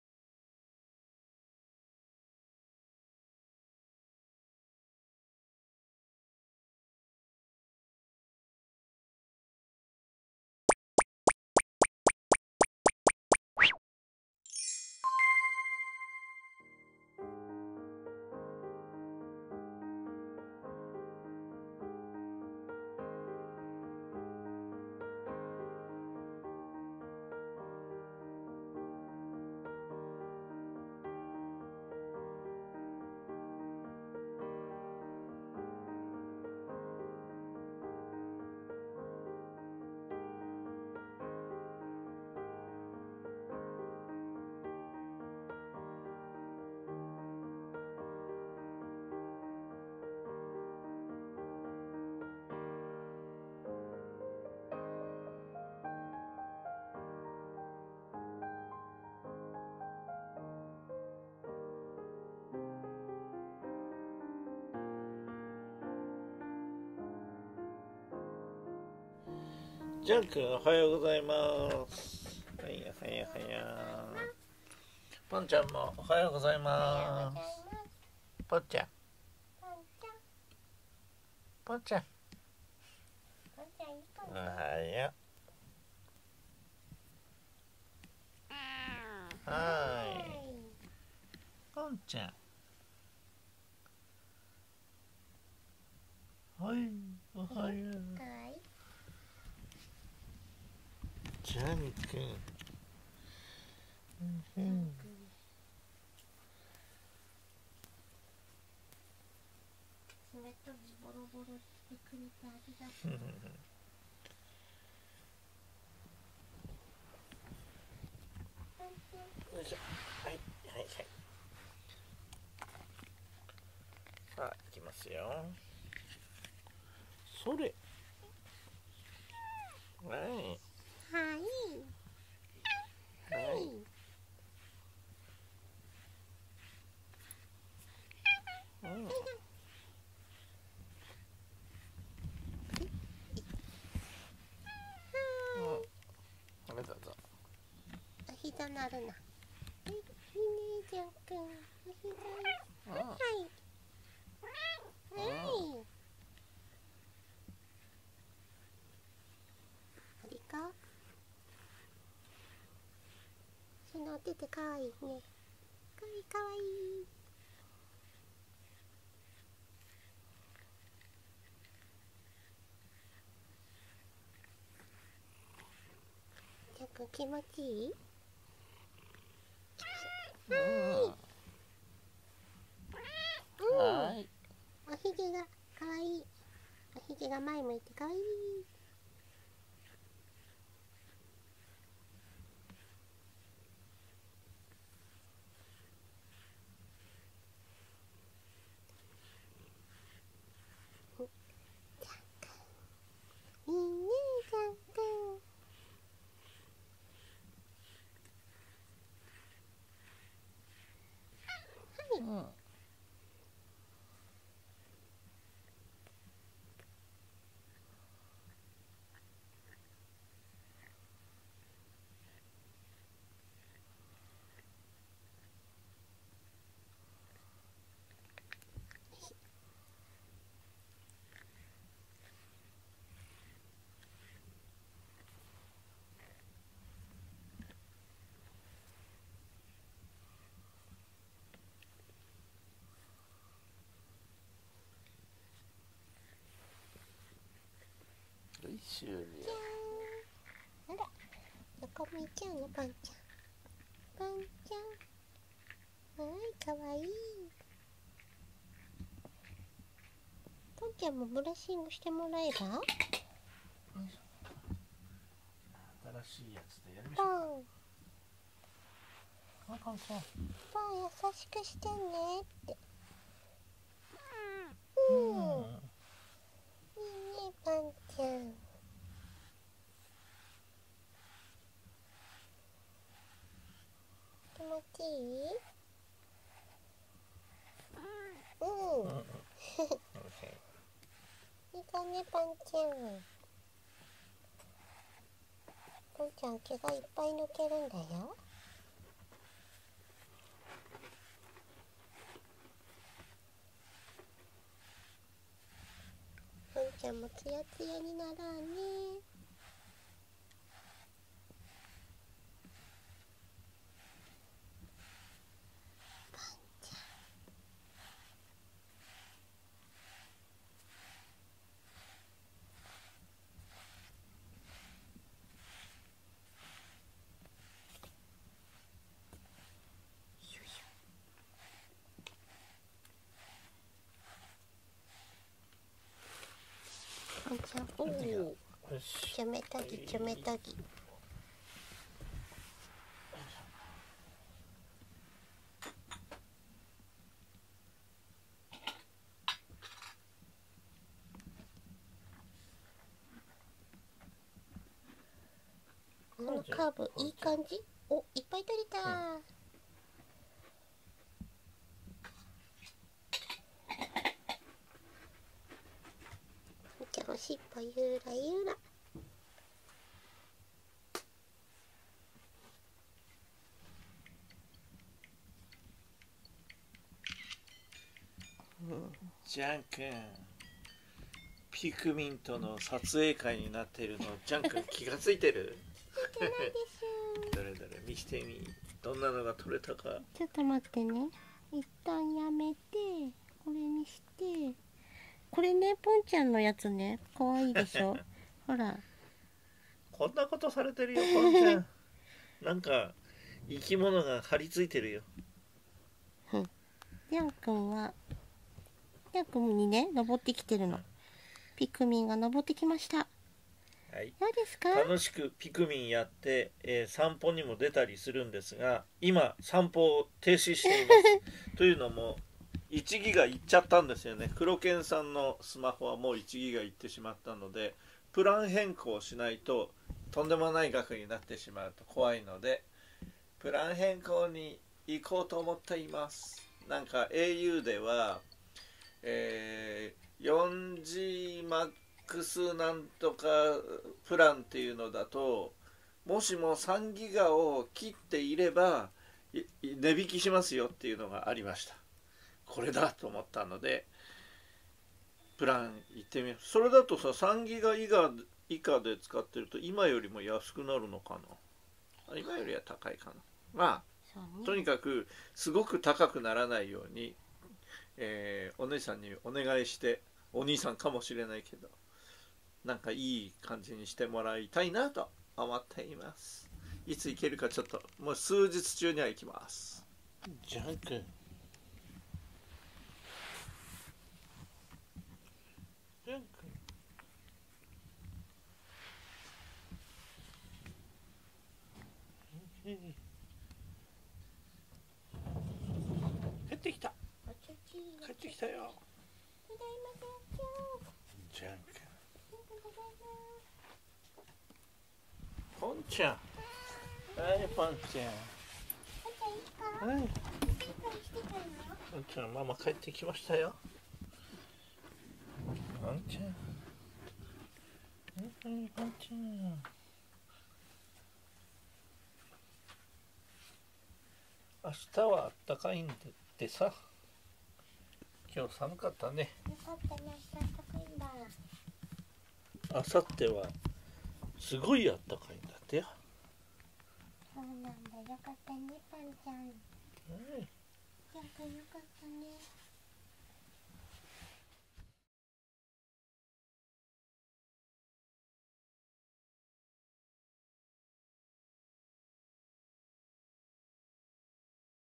パパとママが始めたピクミンはギガを圧迫してる！【Jean & Pont 3129】2023/2/18 保護猫ジャンけんポン
健康になるにはまず歩くことからと始めたピクミンのゲームが、スマホのギガを結構食って１０日で１ギガに到達しました。
この程度は当たり前なのでしょうが、我が家はパパママ共に１ギガのプランでずっと生きて来たのです。
この調子だと大変なことになりそうなのでギガのプランを見直さなければいけません。
それまでは歩けないとパパは言っています。逆効果か〜！